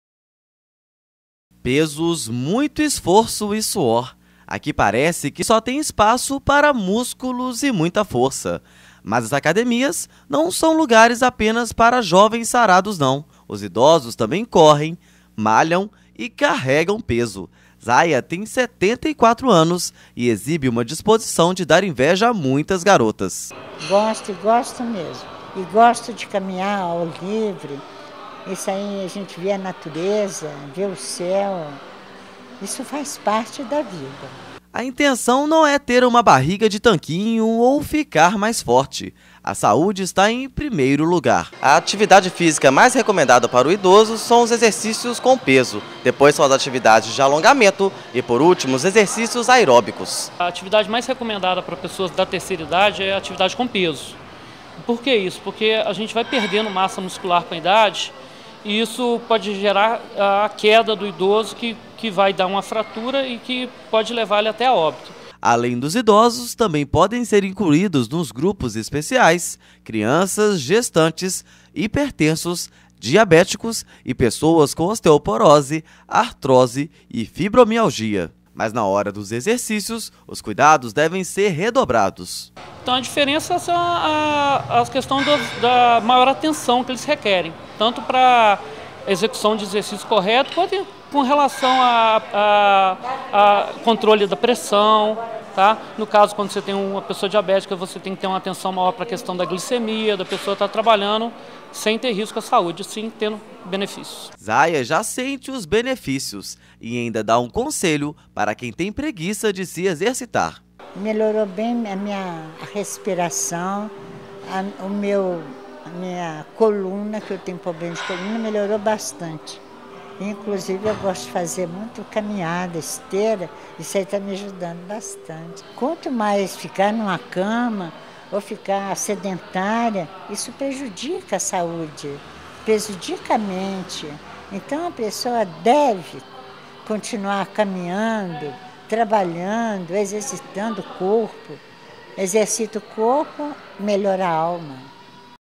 Academia Idosos.avi
Reportagem da TV Viçosa sobre a importância dos idosos praticarem exercícios físicos. A mtaéria aborda também os cuidados necessários para as atividades destinadas a grupos especiais.